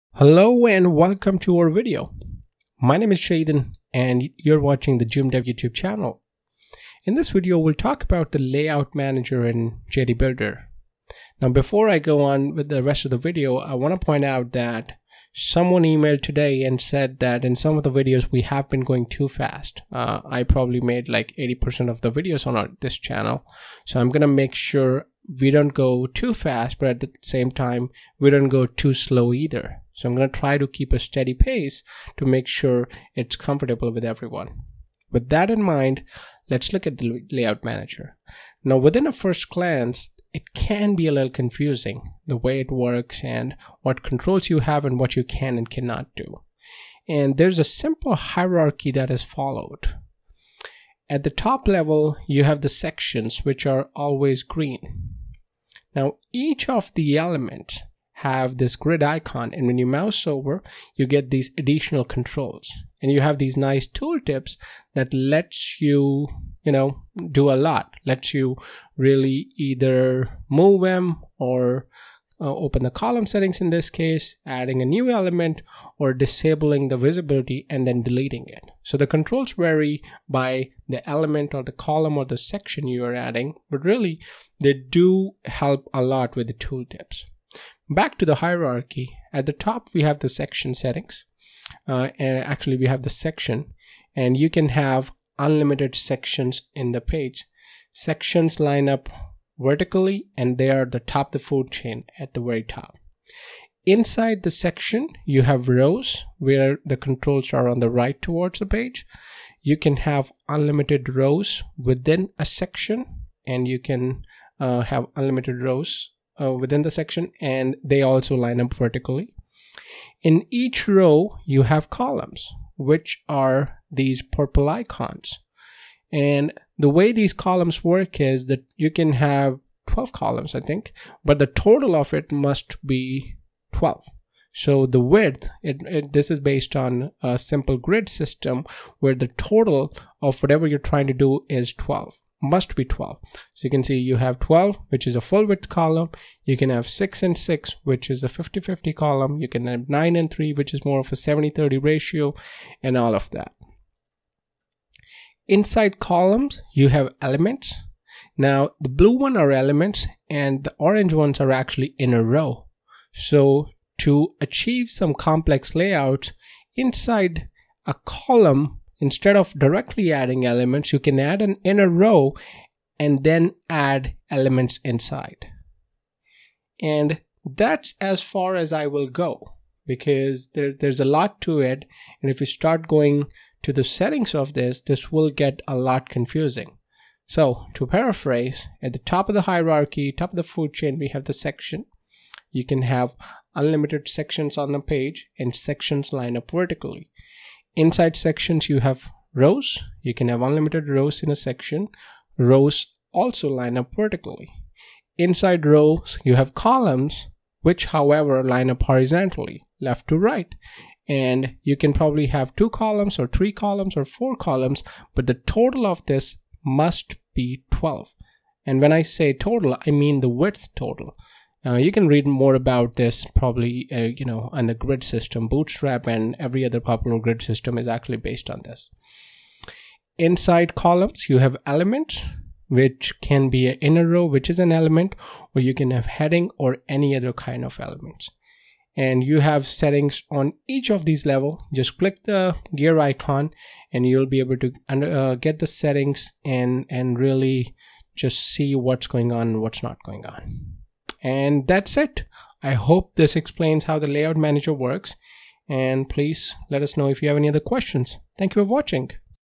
JD Builder Layout Manager for Dummies
There are so many settings at each level that let you modify every design element and possibilities at endless.
The green controls are for section level settings.
The red controls are for row level settings.
The purple controls are for column/inner column level settings.
The orange controls are for inner row level settings (Inner row is just another element that allows you to have better control and create flexible layouts within rows).